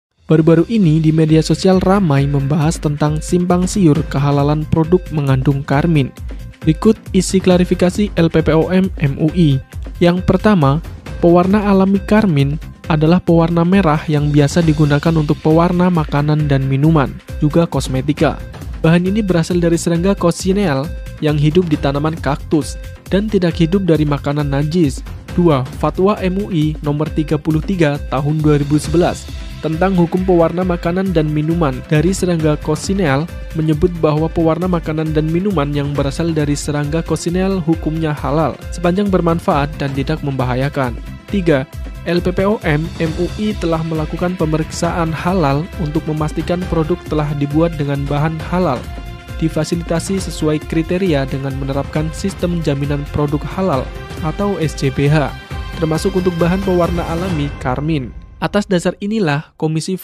KARMIN HALAL ATAU HARAM?
Kemarin-kemarin katanya lagi rame ya kalo karmin itu haram?
tapi kalian tau ga si apa itu karmin?
Karmin adalah pewarna dengan ciri khas merah pekat
biasanya karmin ditemukan hampir pada semua makanan
pewarna karmin terbuat dari serangga tepatnya cochineal kutu daun yang hidup di kaktus dan mengonsumsi kelempaban serta nutrisi makanan.
simak video penjelasan berikut ini sampai selesai ya!